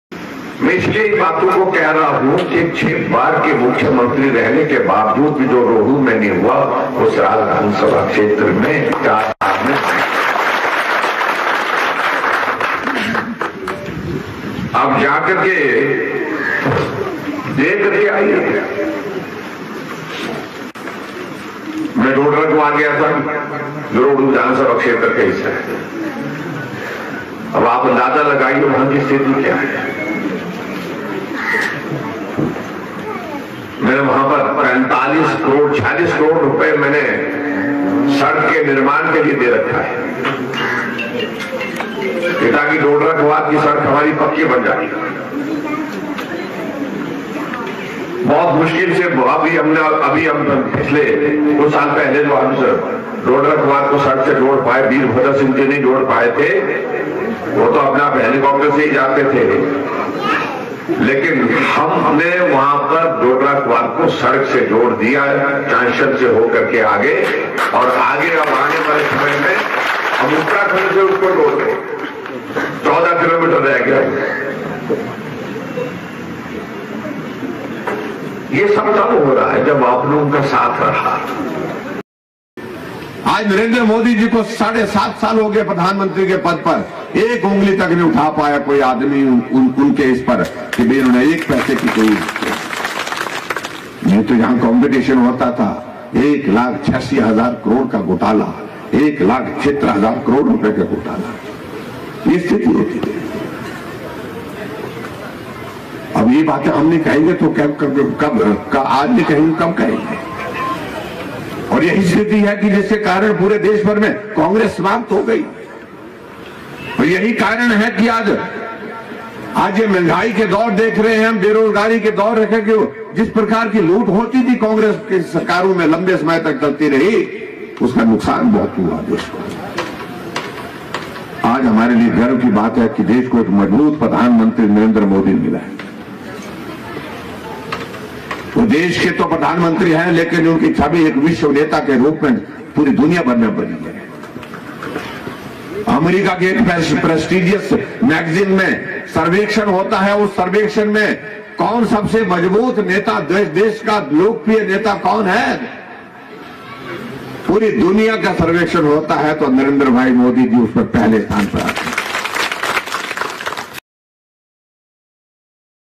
आज देश में महंगाई व बेरोजगारी Congress की देन | Jairam Thakur | Hunger and unemployment | BJP
सीएम जयराम ने कांग्रेस पर निशाना साधा है. मुख्यमंत्री जयराम ठाकुर ने आरोप लगाते हुए कहा कि कांग्रेस पार्टी ने हजारों करोड़ के घोटाले कर देश को नुकसान पहुंचाया है, जबकि इसके विपरीत नरेंद्र मोदी के नेतृत्व में देश विश्व गुरू बनने की ओर अग्रसर है.